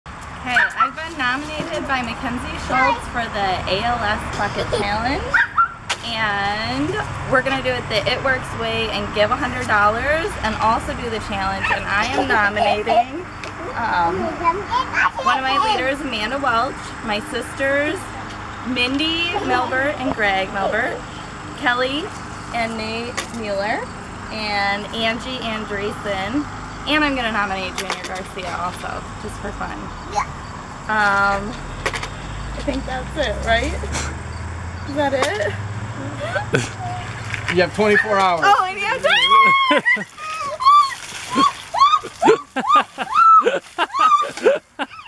ALS Ice Bucket Challenge!
I was nominated by Makenzie Schultz to do the ALS bucket challenge and we decided to do it the It Works! and freeze my bottom off!  I nominate Amanda Lynn Welch, Mindy Andresen Milbert and Greg Milbert, Kelli Andresen and Nate Mueller, Angie Andresen and just for fun one of my awesome neighbors Jr. Garcia!  You have 24 hours to complete the challenge or donate $100 towards ALS research!